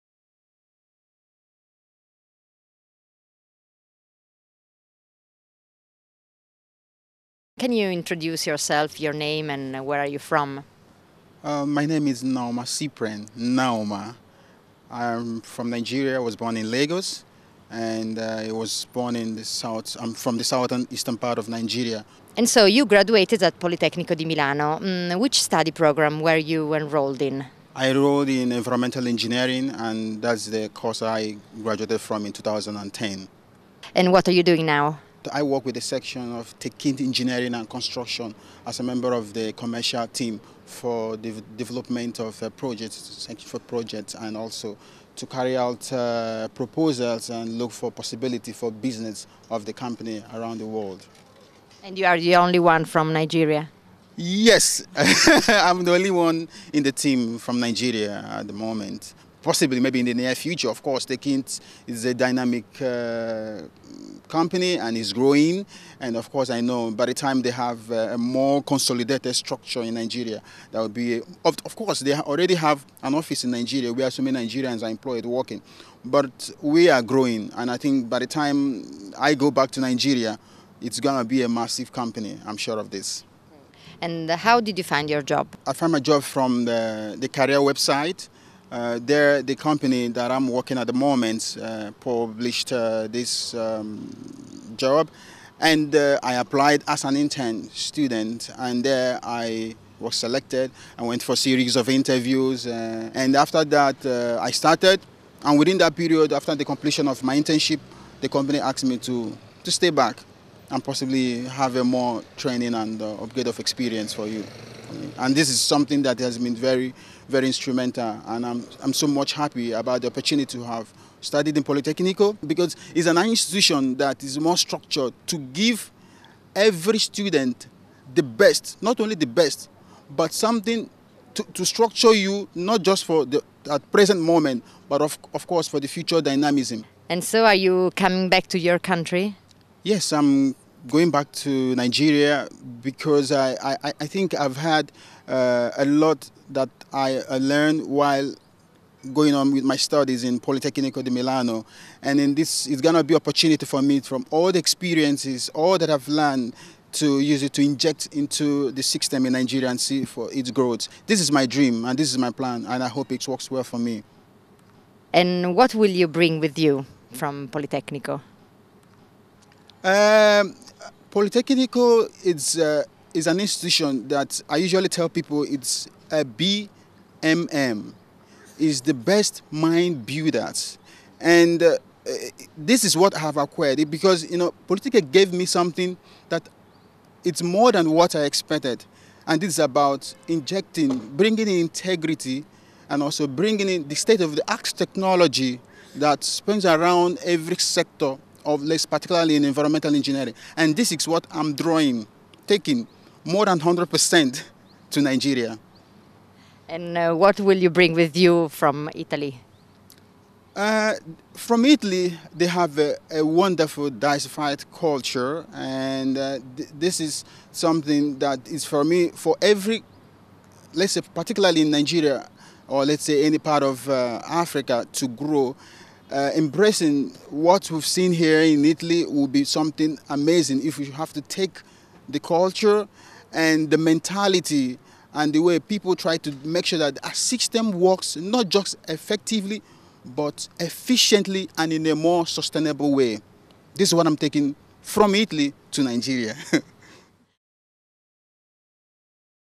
Cyprian Nnaomah Nnaomah (Intervista a)
Intervista a Cyprian Nnaomah Nnaomah, laureato in Environmental Engineering nel 2010.